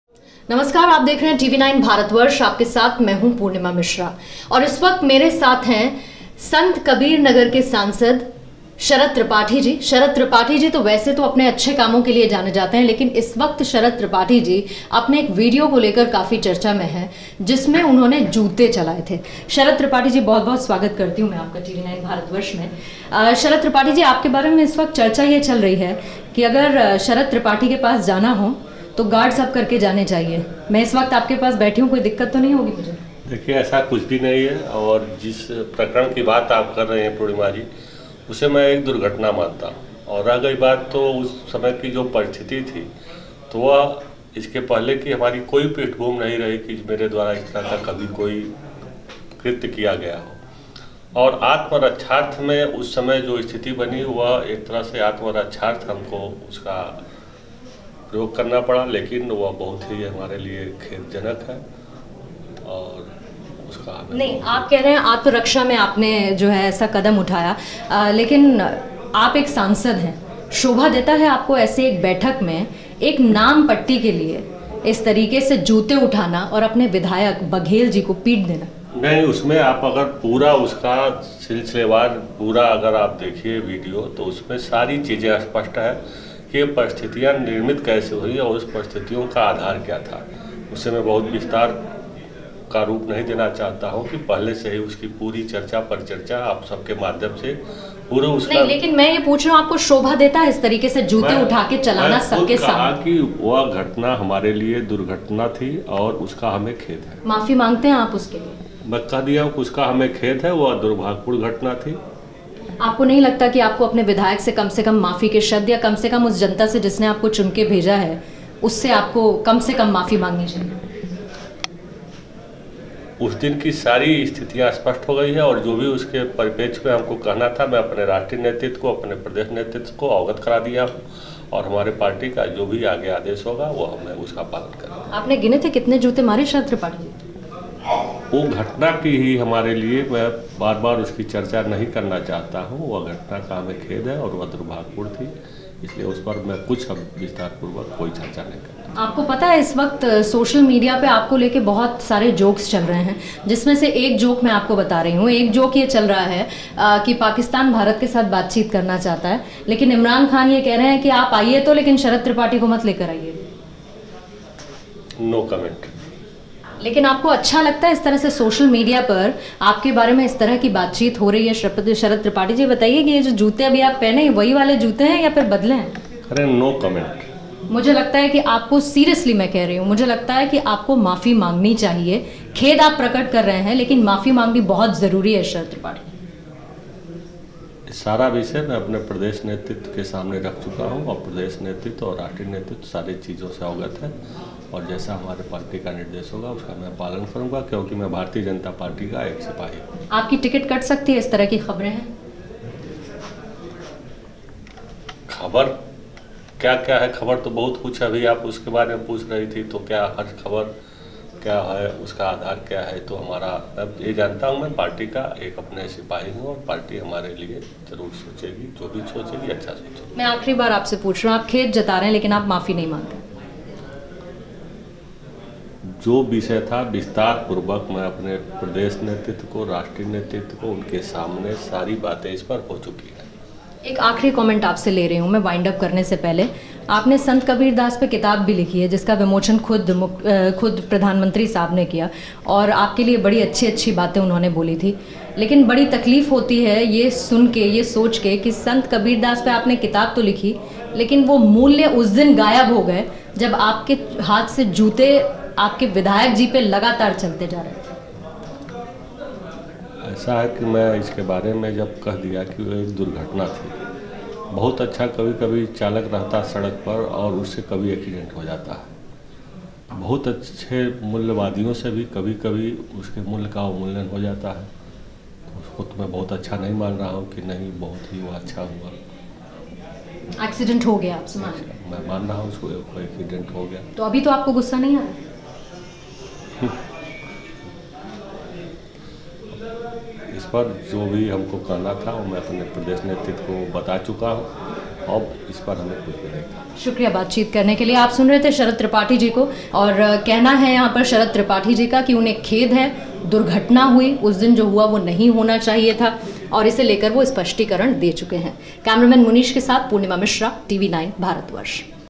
BJP MP Sharad Tripathi Exclusive Interview on Shoe fight with MLA Rakesh Singh Baghel.
Facing flak from every quarters for beating his fellow MLA Rakesh Singh Baghel, Sant Kabir Nagar BJP MP Sharad Tripathi opened up in an exclusive interview with TV9 BHaratvarsh. Defending his actions Tripathi said he clarified his position before UP BJP chief Mahendra Pandey. CM Yogi Adityanath reportedly shot a letter to Amit Shah requesting not to give Tripathi another chance in the upcoming Lok Sabha election Watch LiveTV